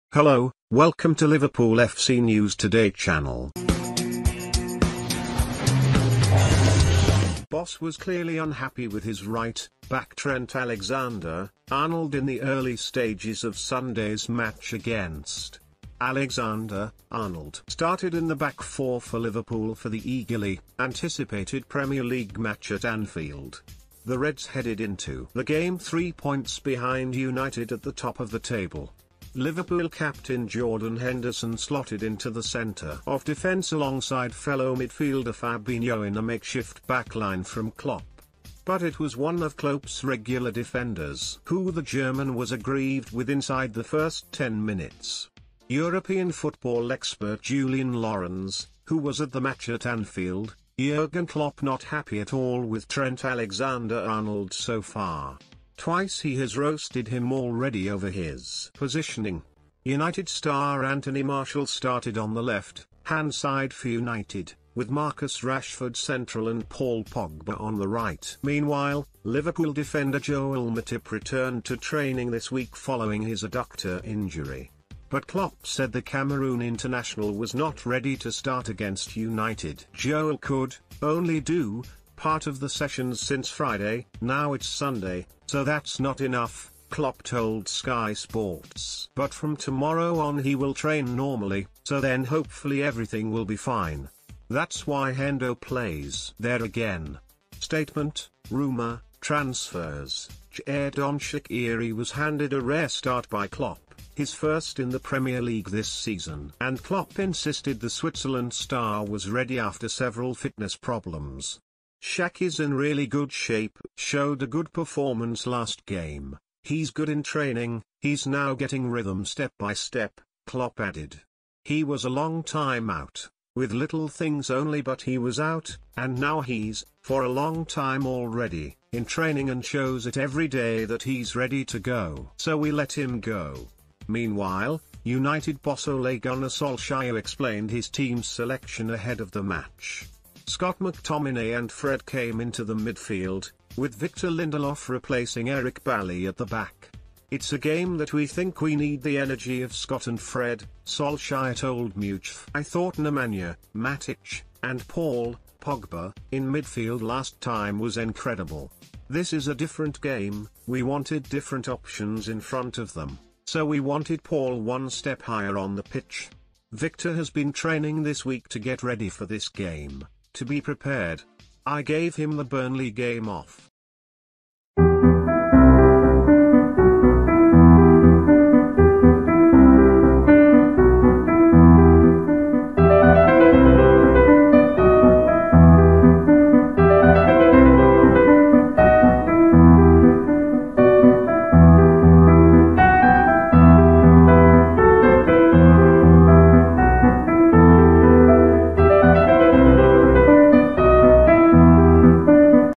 Jurgen Klopp 'roasts' Trent Alexander-Arnold during Liverpool and Man Utd clash - news today
boss was clearly unhappy with his right-back Trent Alexander-Arnold in the early stages of Sunday's match against . Alexander-Arnold started in the back four for Liverpool for the eagerly-anticipated Premier League match at Anfield. The Reds headed into the game three points behind United at the...